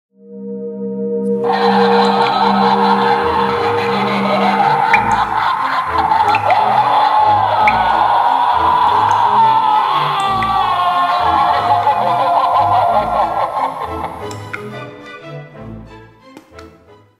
Flying Animated Vampire Bat By Mark Roberts
Something spooky is coming your way this Halloween with this animated flying vampire bat. The red eyes glow as he crackles something scary at you.